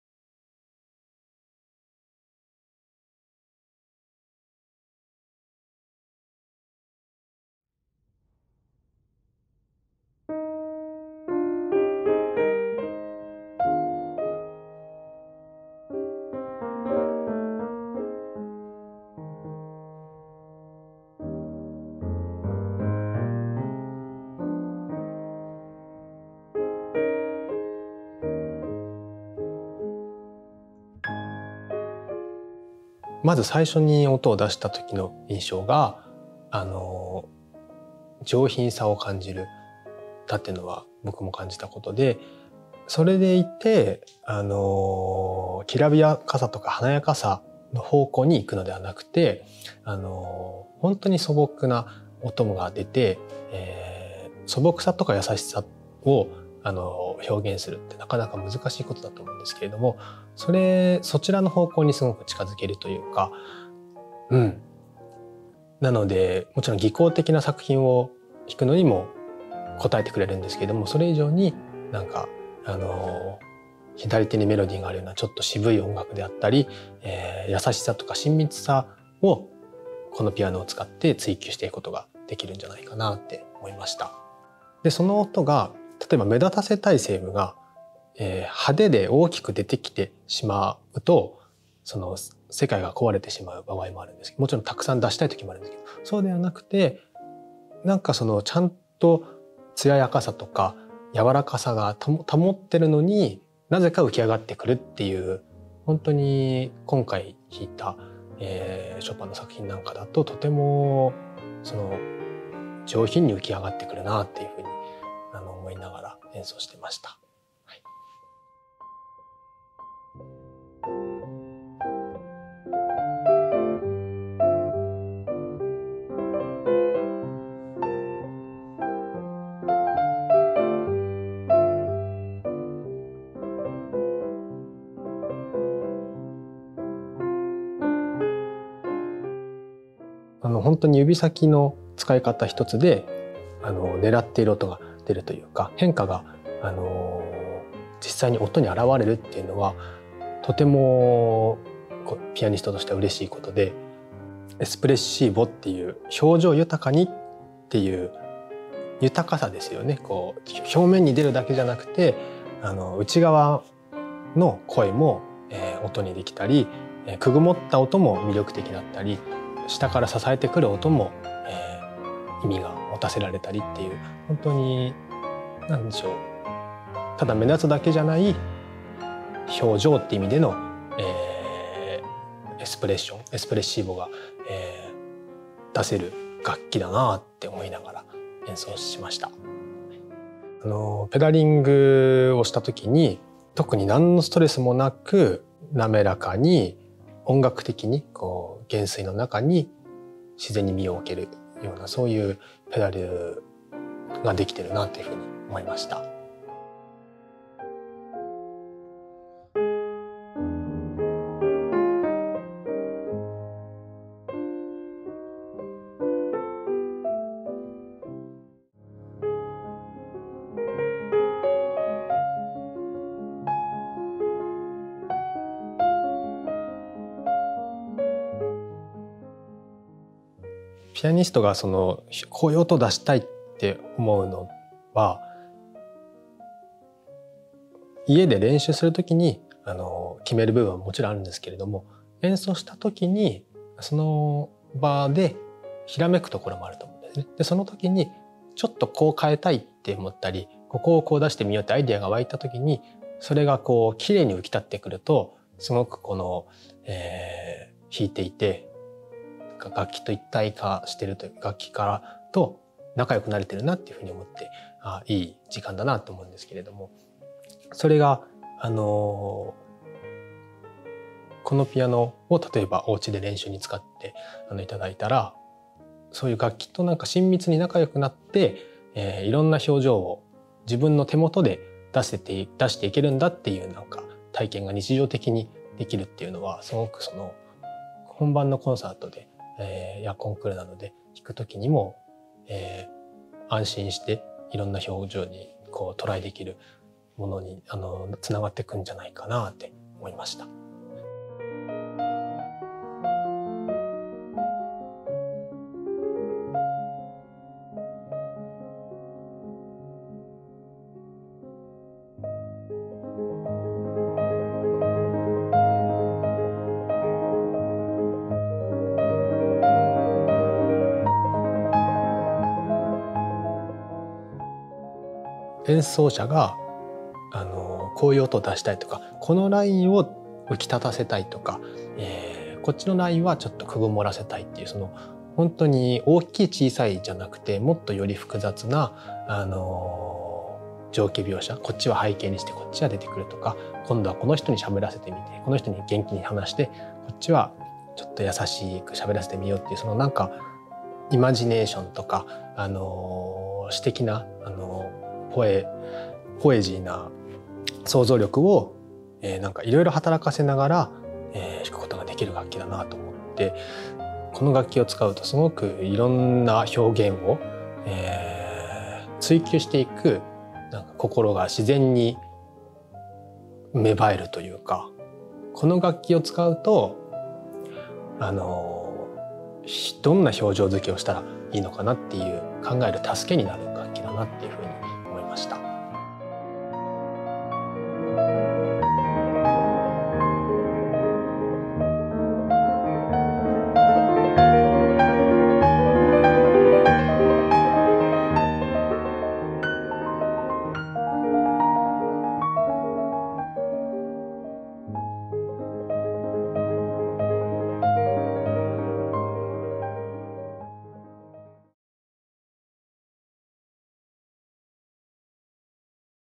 實川風が語るC3X espressivoの魅力
国内外での演奏活動を広げている若手ピアニスト 實川 風さんが新発売のC3X espressivoの魅力について語ってくれました。

■實川 風（じつかわ かおる)　プロフィール
2015年パリのシャンゼリゼ劇場で行われたロン・ティボー・クレスパン国際コンクールにて、第3位(1位なし)、最優秀リサイタル賞、最優秀新曲演奏賞を受賞。
2016年、イタリアで行われたカラーリョ国際ピアノコンクールにて第1位・聴衆賞を受賞。
現在、日本の若手を代表するピアニストの一人として、国内外での演奏活動を広げる。